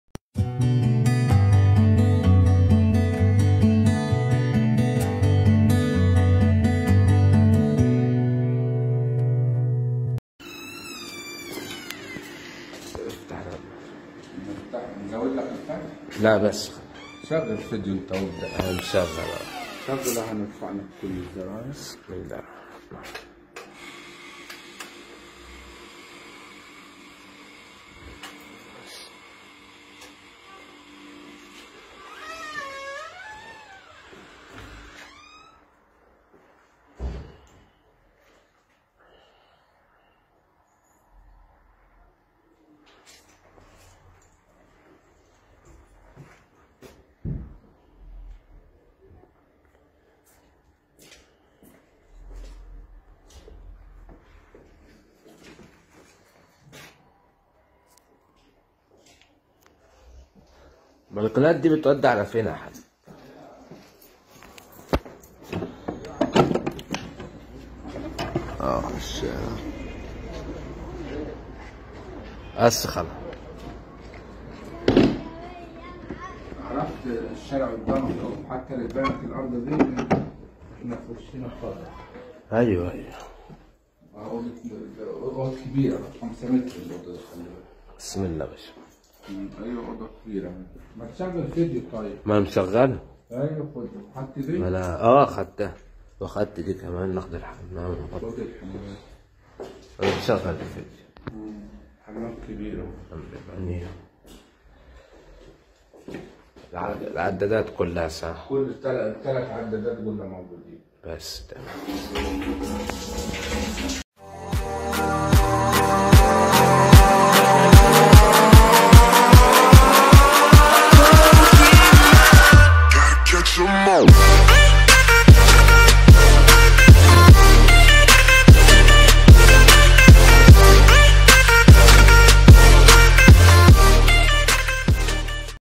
اعلانات شقق للبيع شقة تمليك شارع اسكندر ابراهيم 110 م 3 غرف وريسبشن 3 عدادت سوبر لوكس
شقه تالت نمره من شوارع اسكندر ابراهيم 110 متر 3 غرف وصاله قطعتين منهم غرفتين عا الشارع جميع العدادات دور3 للاستعلام.01289931249 مطلوب 600 الف كاش